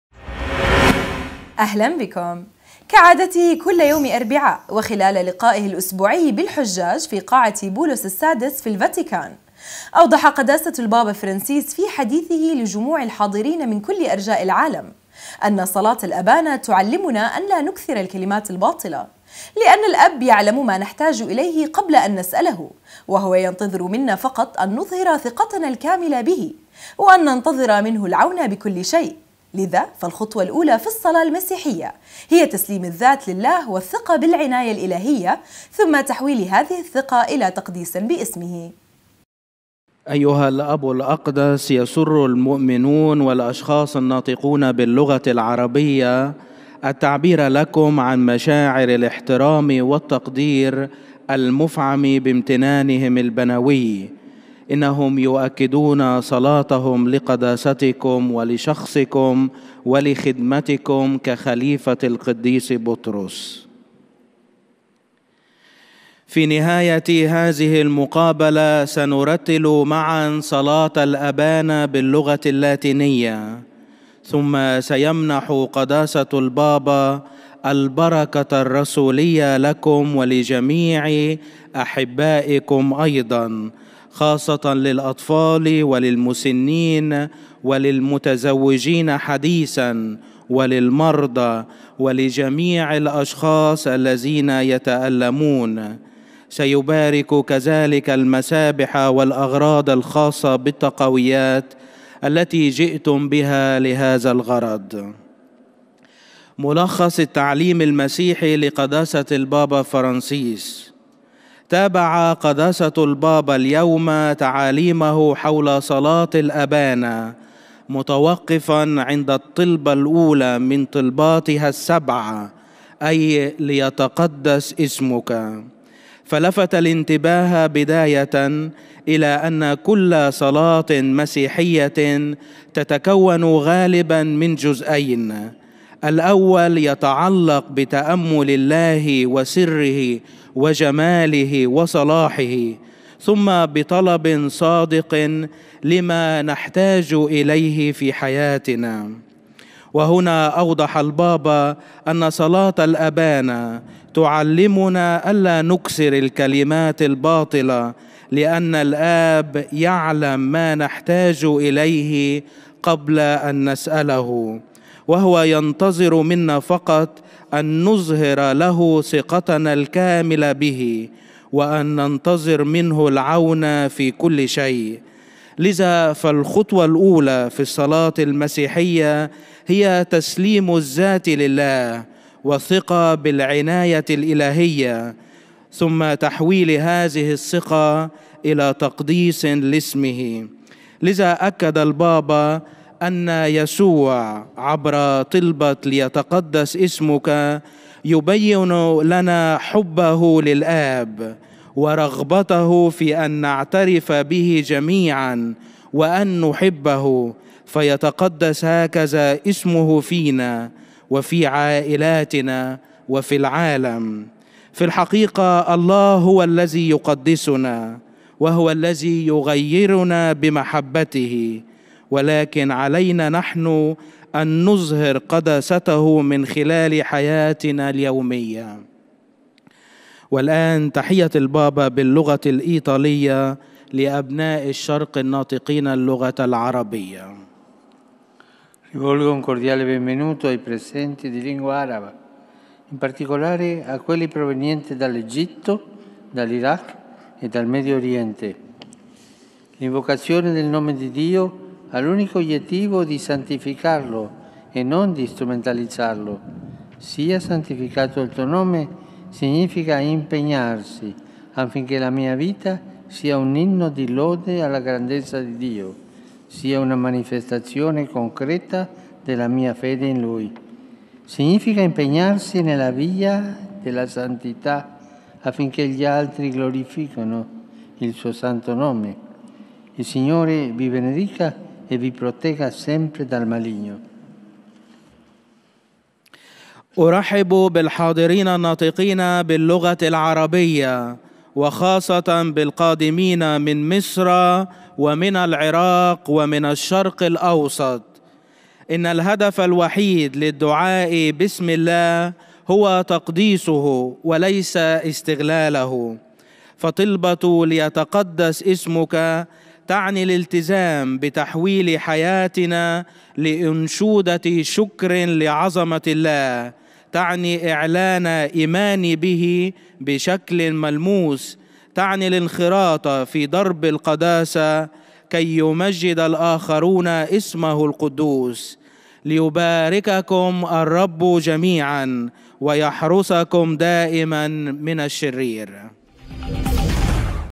نورسات الأردن : البابا فرنسيس : صلاة الابانا تعلمنا ان لا نكثر الكلمات الباطله
كعادتهِ كلَ يومِ أربعاء / وخلالَ لقائهِ الاسبوعي بالحجاجِ في قاعة بولس السادس في الفاتيكان /، أوضحَ قداسةُ البابا فرنسيس في حديثهِ لجموعِ الحاضرينَ منْ كلِ أرجاءِ العالمْ /، أن صلاة الابانا تعلمنا ان لا نكثر الكلمات الباطلة / ، لأن الاب يعلم ما نحتاج اليه قبل ان نسأله/، وهو ينتظر منا فقط ان نظهر ثقتنا الكاملة به / وان ننتظر منه العون بكل شيء/ ، لذا فالخطوة الأولى في الصلاة المسيحية /هي تسليم الذات لله والثقة بالعناية الالهية /ثم تحويل هذه الثقة الى تقديس بإسمه/ .